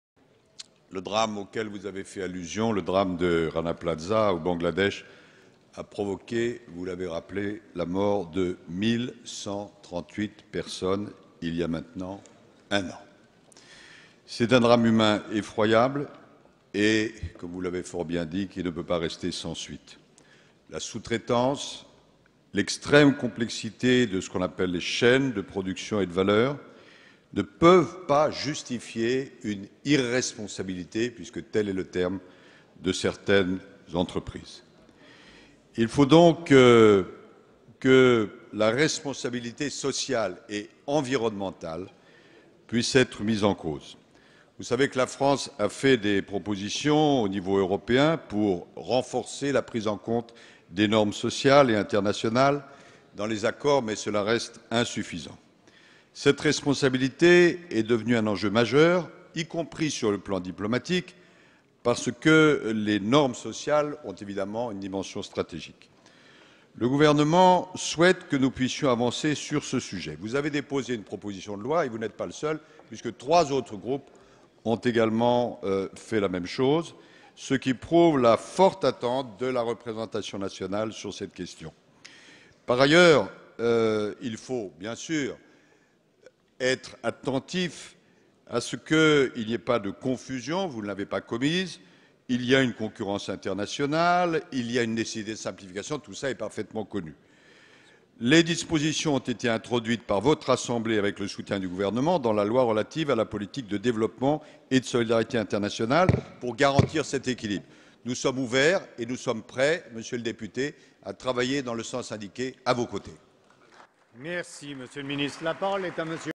Drame du Rana Plaza (Bangladesh) : Laurent Fabius répond à une question à l'AN (06/05/2014)
Laurent Fabius, ministre des Affaires étrangères et du Développement international, a répondu à une question à l'Assemblée nationale à propos du drame de Rana Plaza à Dacca, capitale du Bangladesh et du fonds d'indemnisation pour les victimes. Le ministre a rappeler le devoir de vigilance des multinationales.

Instragram : @francediplo